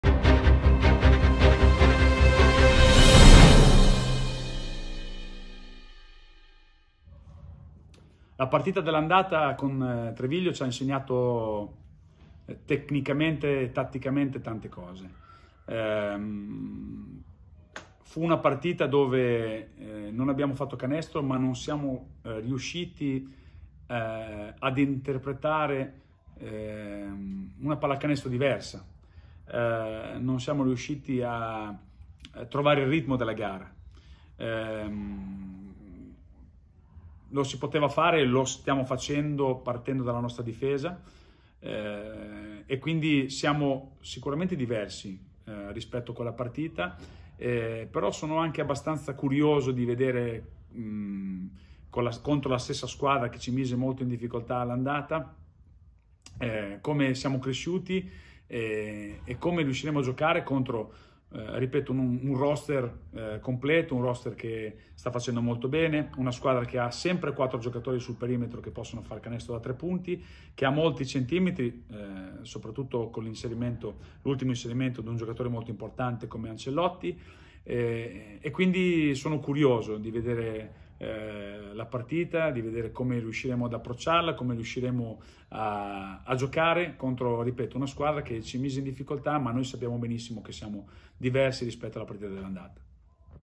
Coach Cavina presenta Reale Mutua Torino - Treviglio
Coach Cavina presente la difficile sfida con Blu Basket Treviglio 💛💙